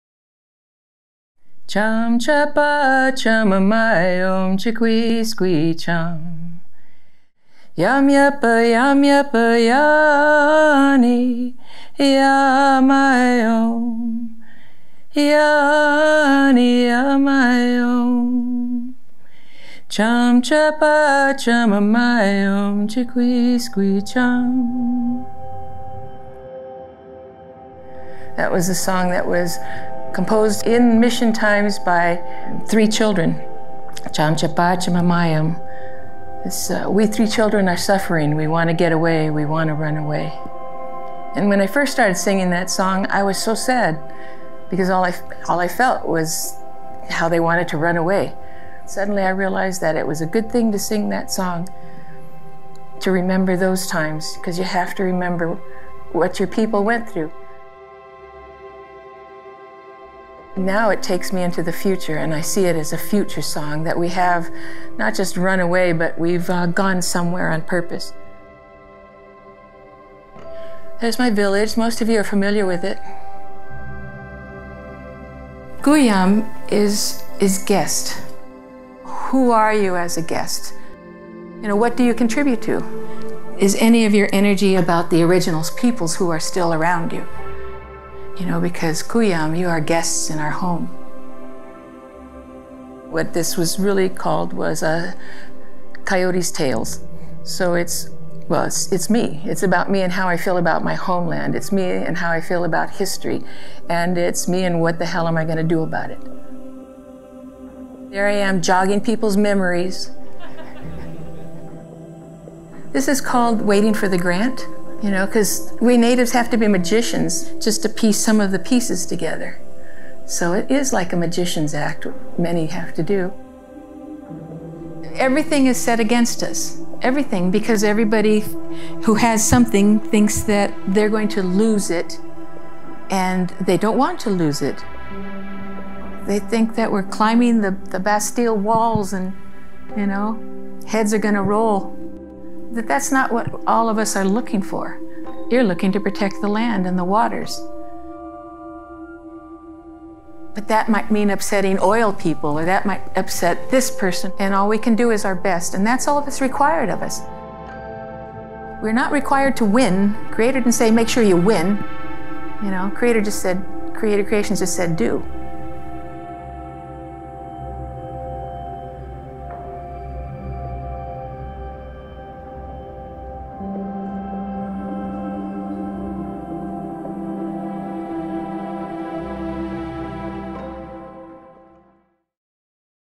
Lifes: L. Frank
Excerpt from a lecture by Tongva-Ajachmem artist, writer, tribal scholar, cartoonist, and community activist L. Frank on MArch 22, 2022. In conjunction with the exhibition Lifes.